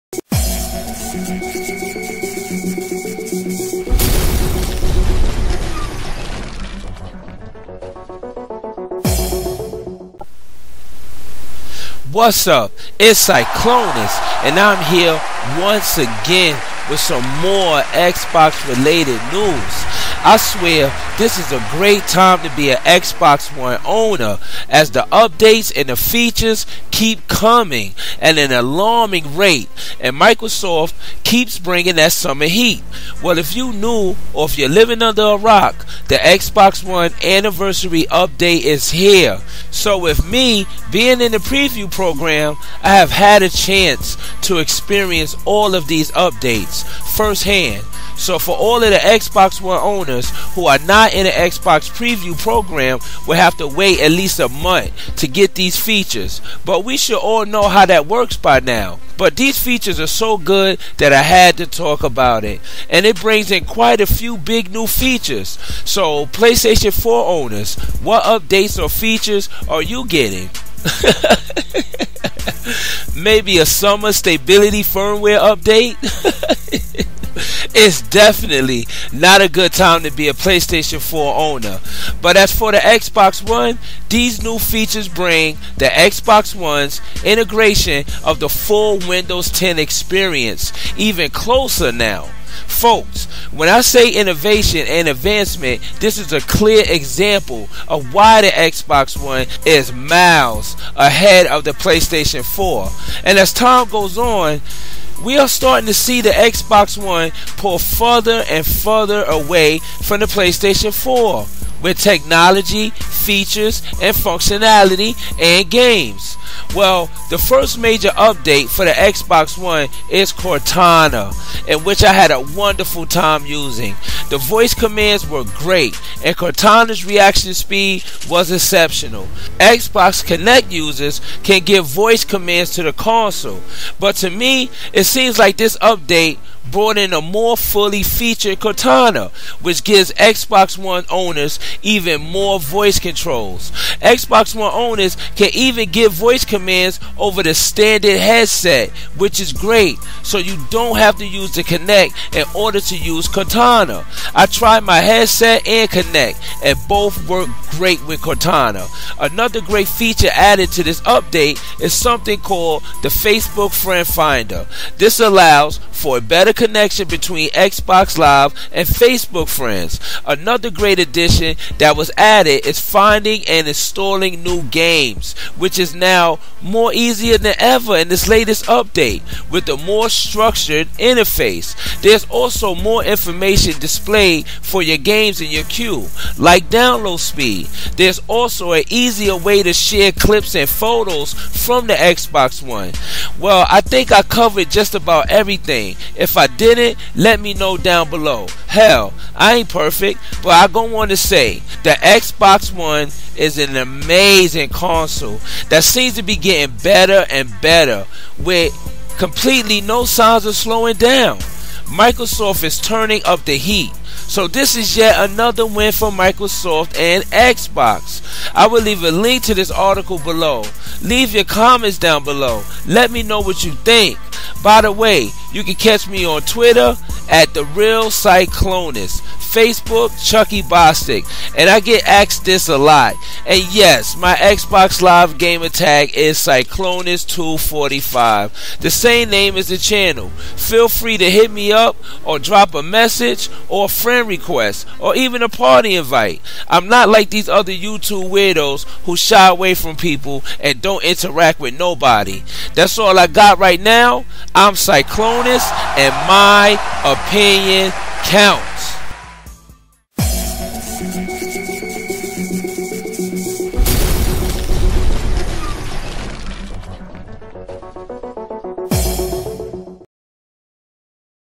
Windows 10 Anniversary Update Makes The Xbox One Even Better
This is the biggest update to the Xbox One since the November update that brought the new dashboard and upgraded the console to essentially run the Windows 10 operating system. The Win 10 Anniversary update will add even more features to bring more interoperability between the console and PC, plus a bevy of new features.
Xbox Live GT : Cyclonis245